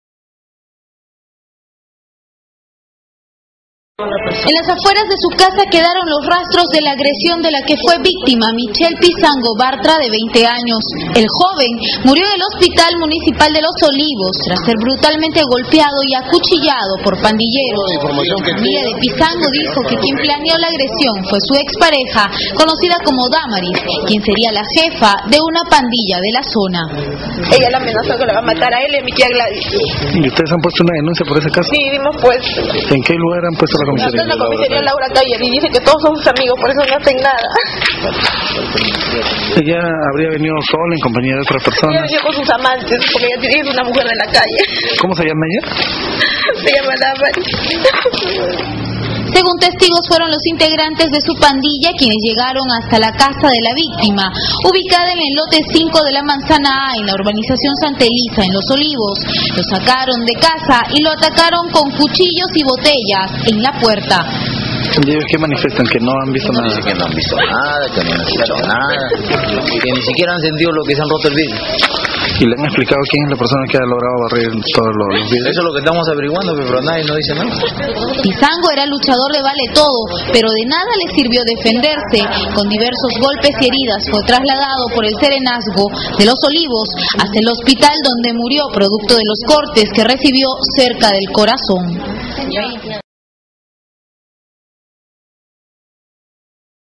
El joven Michael Pizango fallece luego de ser acuchillado por una pandilla
El joven, quien en marzo cumplió 20 años, era amante de las artes marciales mixtas y los deportes de contacto; en su récord figura varias luchas de submission wrestling, dos peleas amateurs y una pelea profesional en artes marciales mixtas, una pelea en muay thai amateur y se estaba preparando para boxear en los "Guantes de Oro" que se realizaba a fines del mes de mayo.

En nombre de todo el equipo de FCPERU.COM nuestras más sentidas condolencias a sus familiares, a sus amigos, a sus entrenadores y a todos los que lo conocieron de cerca.